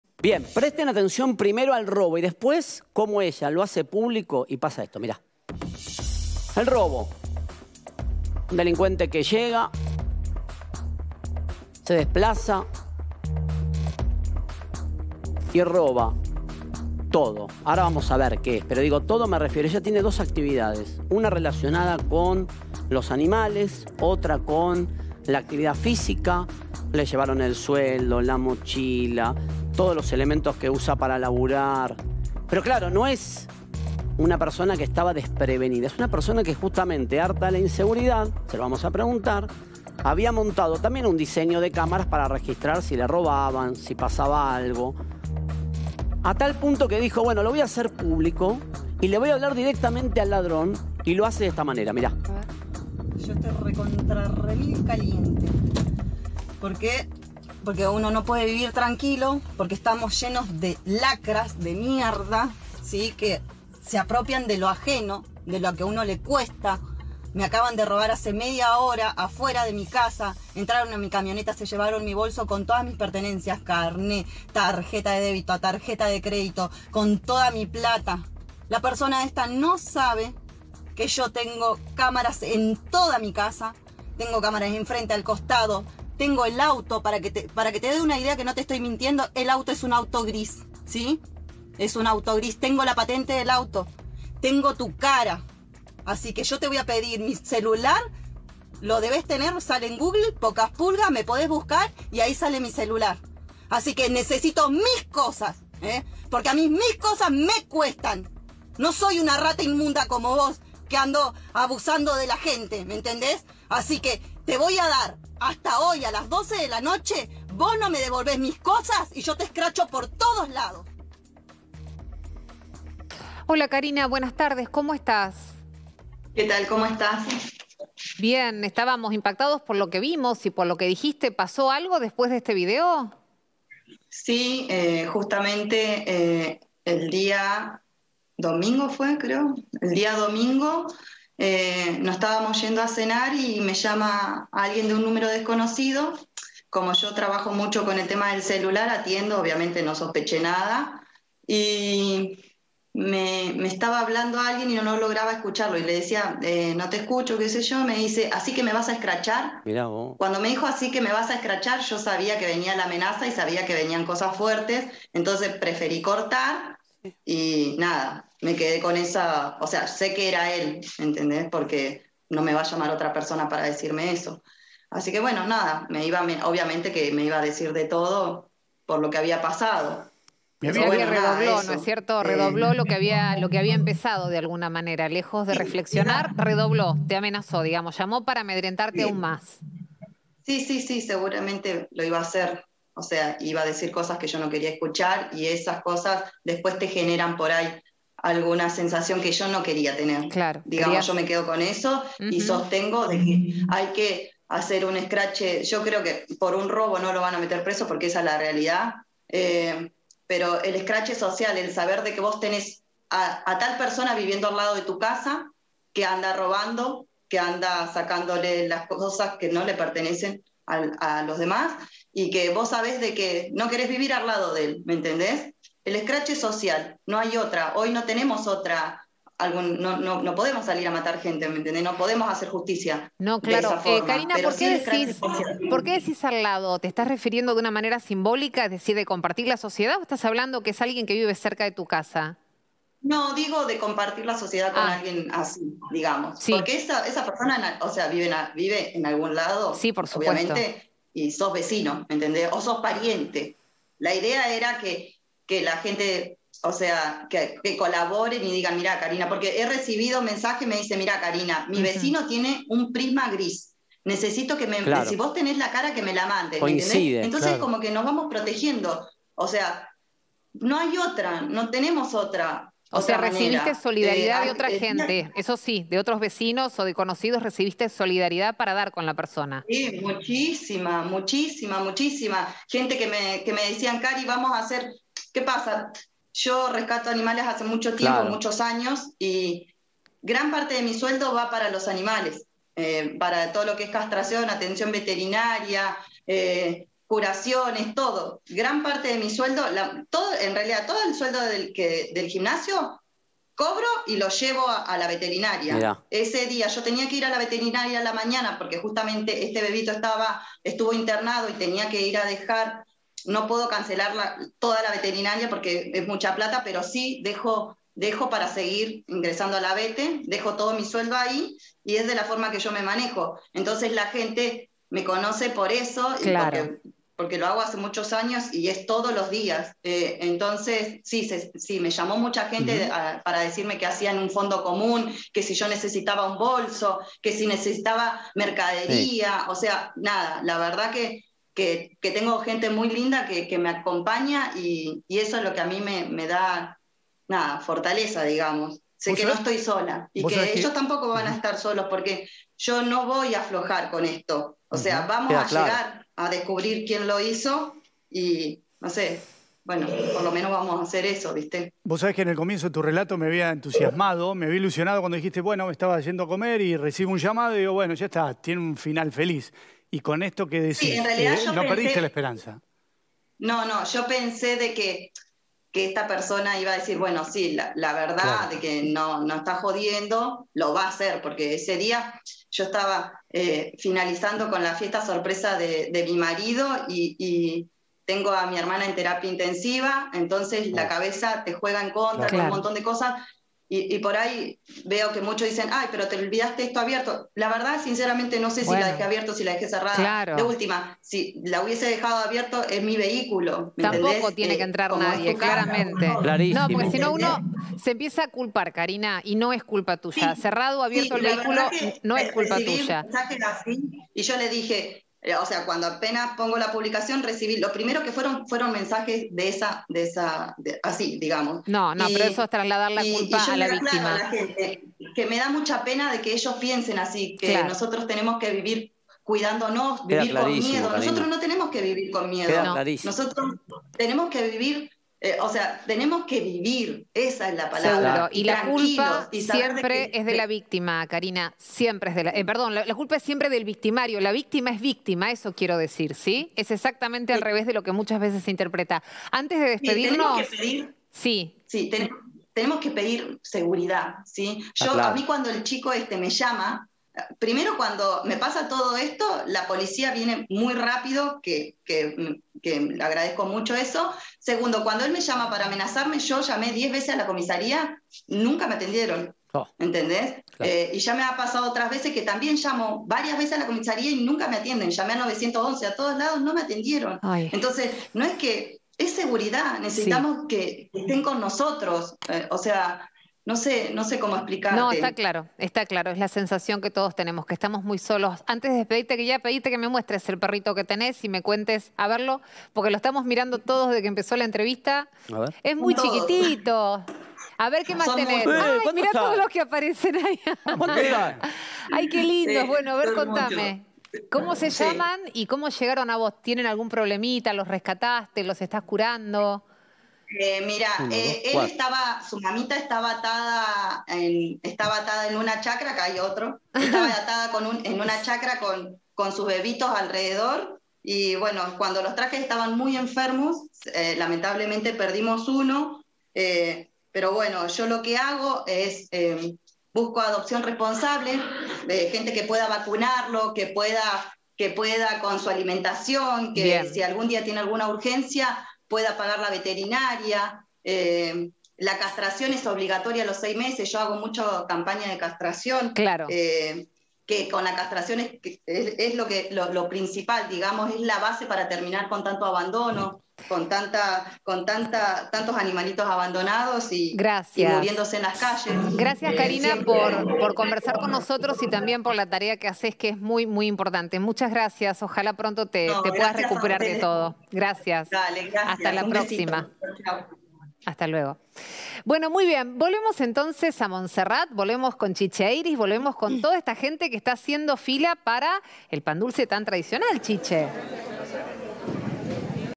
Dejó el sueldo en el auto y se lo robaron
Carina dejó su bolso en el auto y vio a través de las cámaras quién le robó. El Noti de la Gente habló con ella para saber más de su situación.